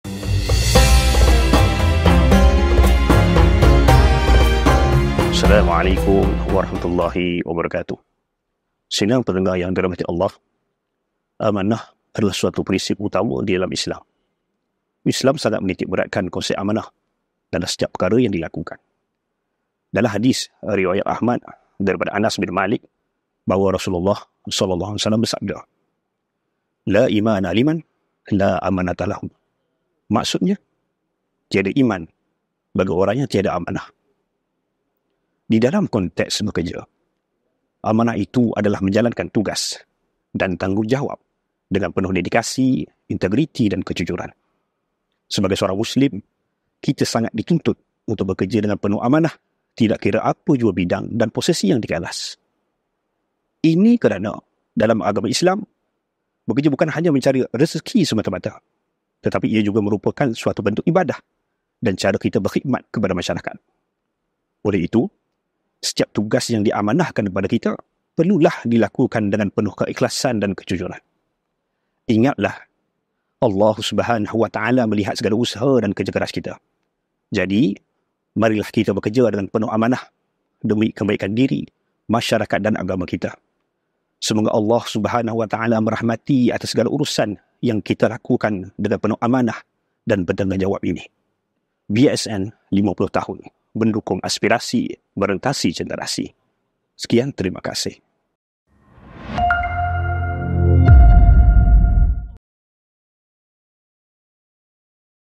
TazkirahBSN Ep 5 Amanah dalam bekerja
Bersama Ustaz Ahmad Fathi Bin Noor Suria, Pegawai Hal Ehwal Islam JMWP.

Dalam sesi perkongsian Tazkirah BSN x Jabatan Mufti WP menerusi tajuk "Amanah Dalam Bekerja".